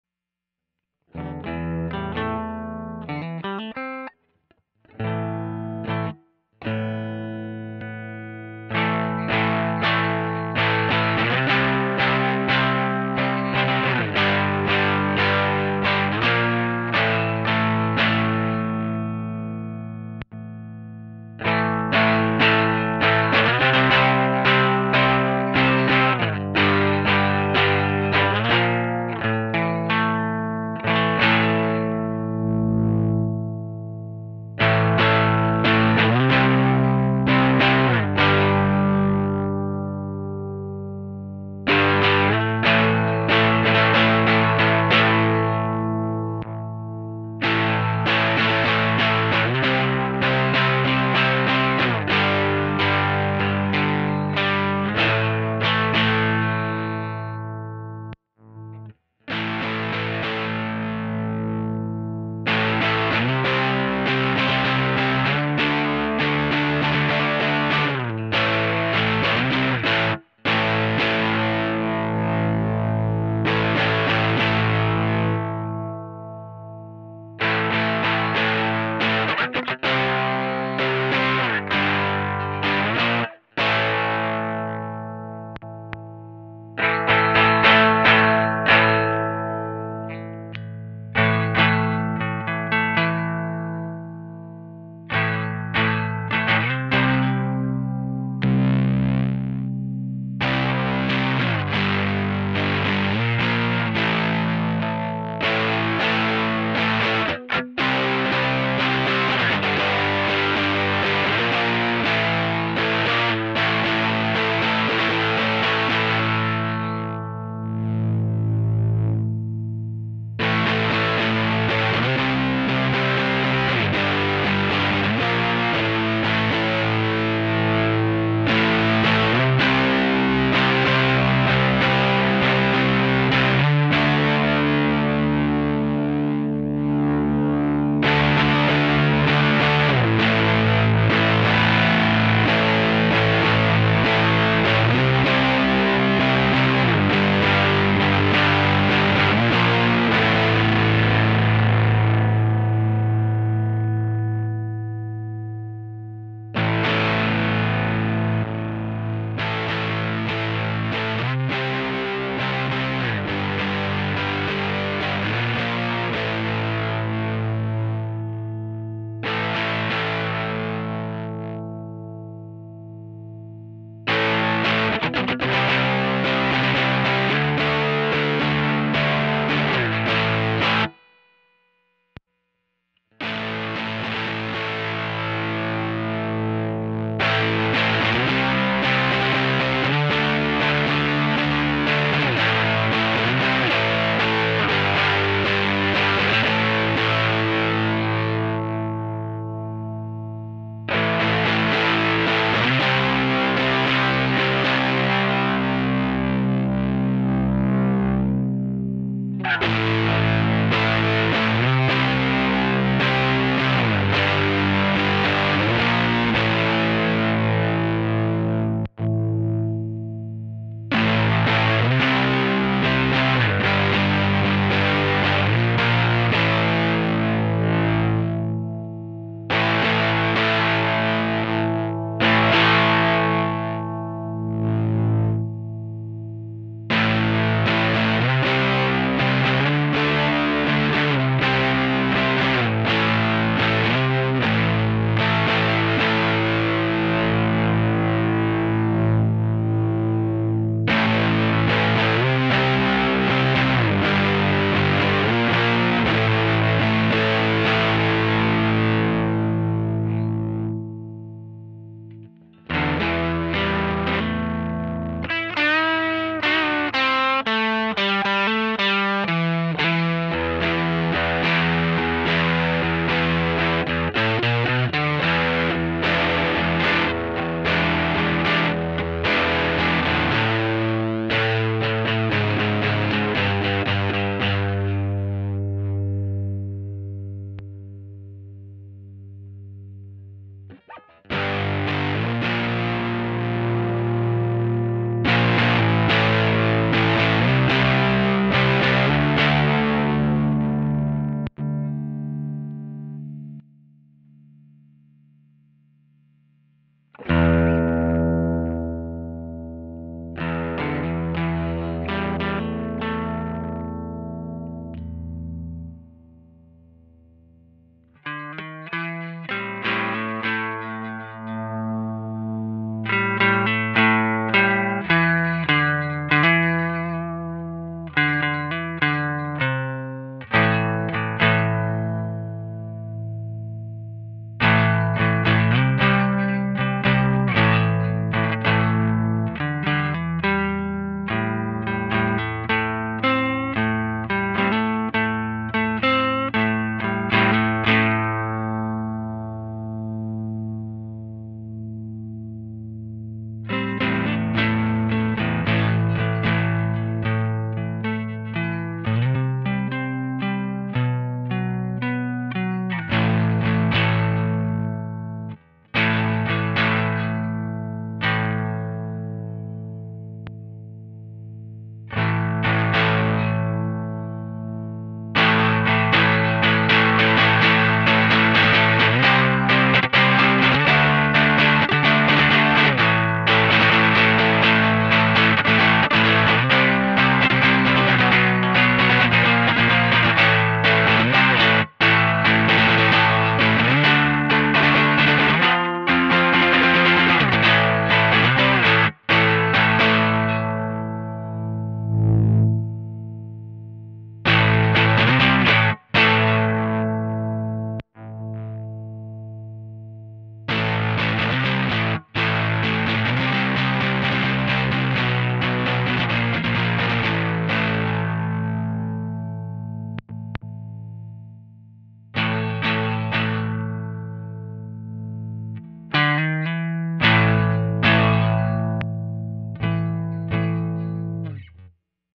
Hudson Broadcast clone with Voltage Adjustment 9V 18V 27V
Discrete Class A Germanium Preamplifier. Runs on regular 9volt power supplies (voltage is increased internally). Vid is recorded with pedal plugged into iphone adapter. Sounds stunning when pushing a real amp. Available at reverb dot com